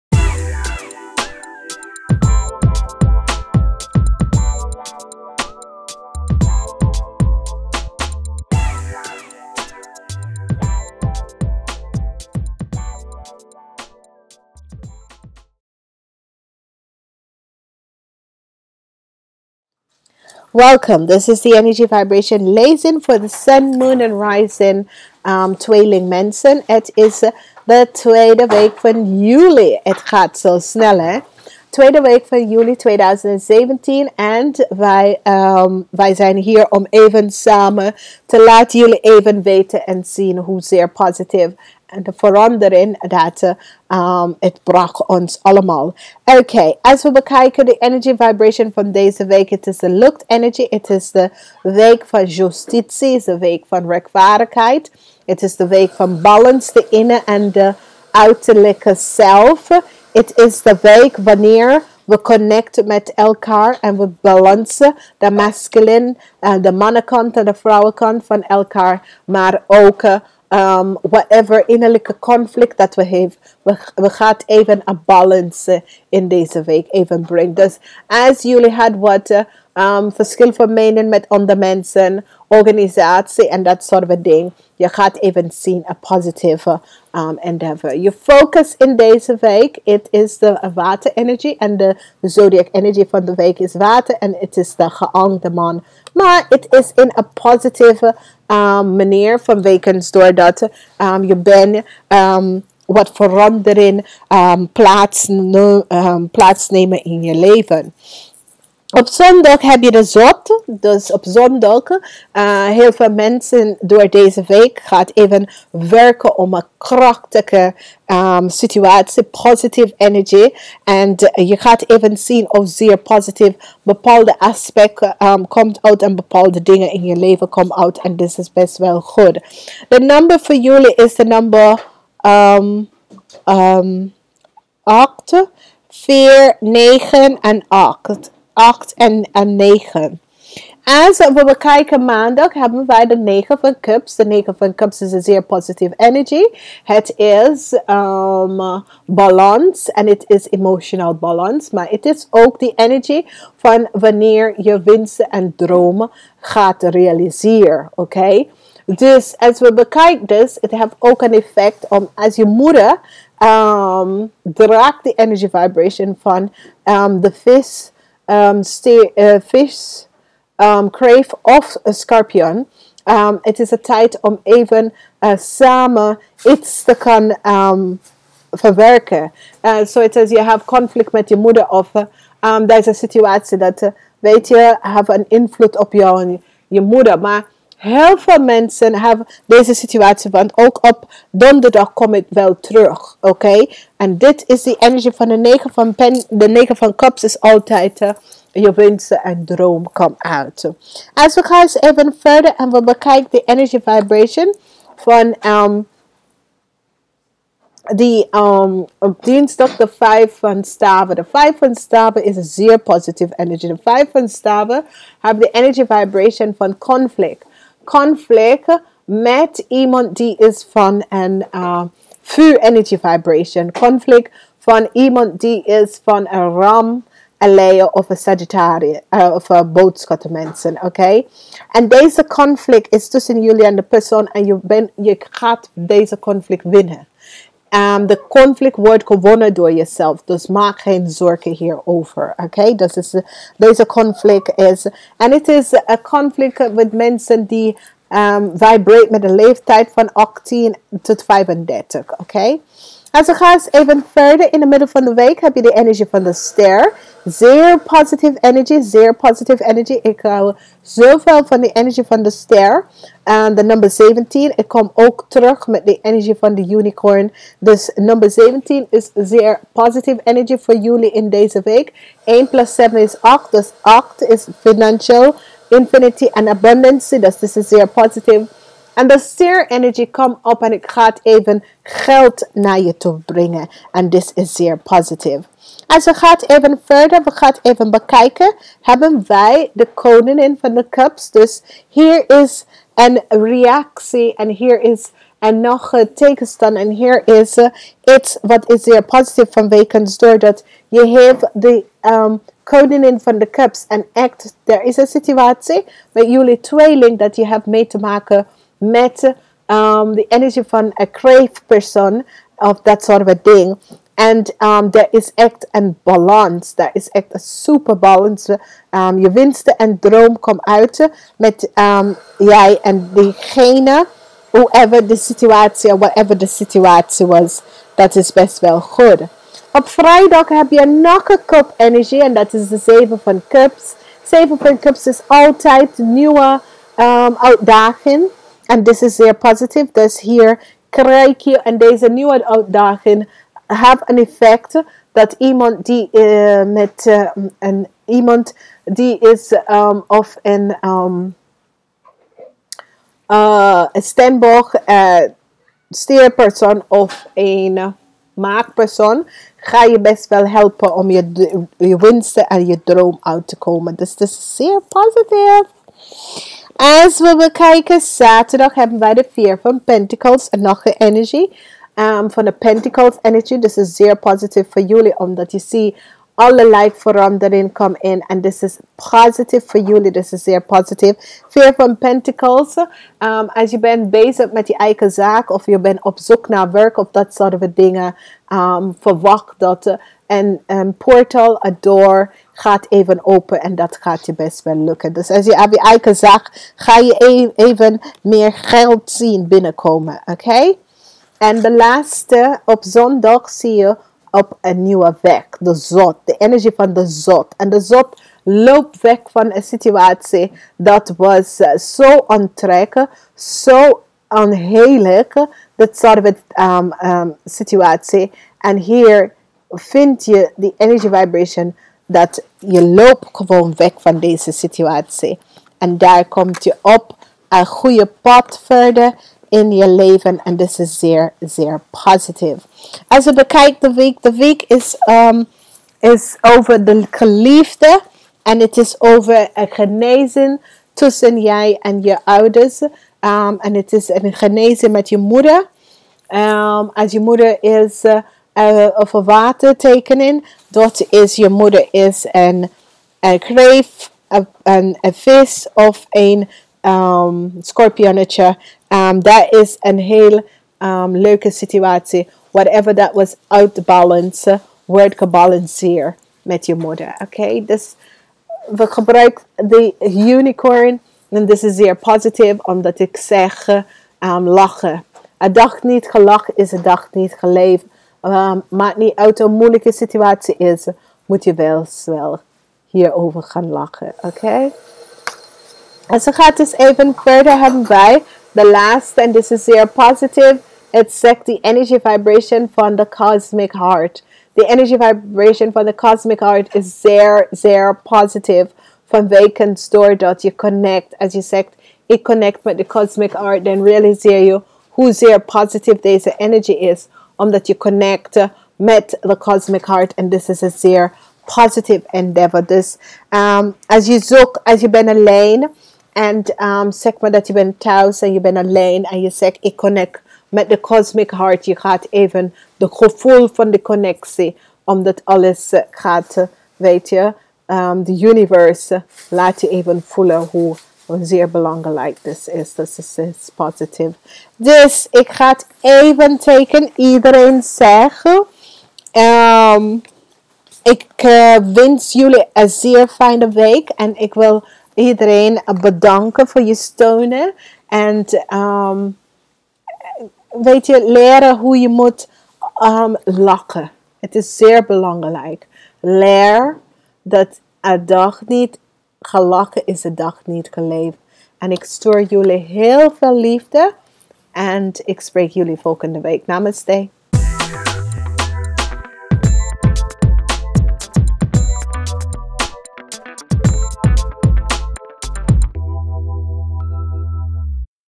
Tweelingen Wekelijkse Tarot Kaartlegging Horoscoop Lezing | Week 28 | 10 Juli t/m 16 Juli
Wekelijkse Tarot Kaartlegging Horoscoop Numerologie voor alle Sterrenbeeld 10 Juli  t/m 16 Juli 2017